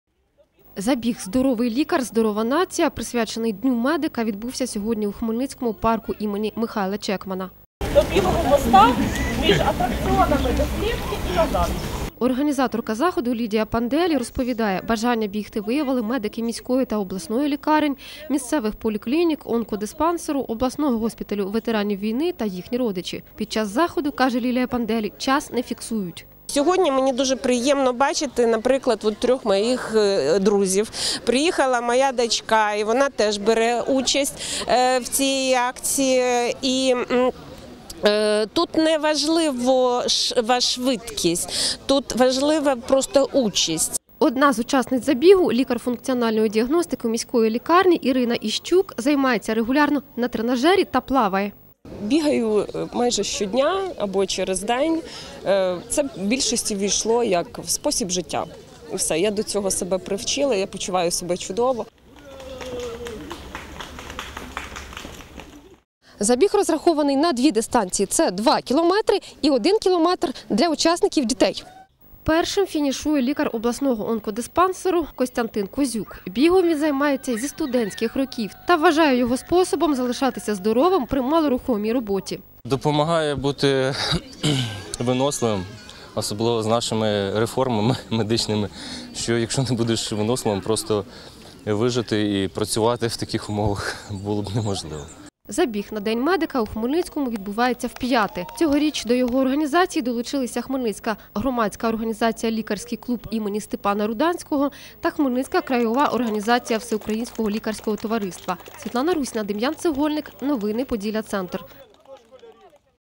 30 хмельницьких медиків взяли участь у забігу «Здоровий лікар – здорова нація»
Забіг «Здоровий лікар – здорова нація», присвячений дню медика, відбувся сьогодні у хмельницькому парку ім. Михайла Чекмана. Цьогоріг участь у ньому взяли медики обласної та міської лікарень, місцевих поліклінік, онкодиспансеру , госпіталю ветеранів війни та їхні родичі.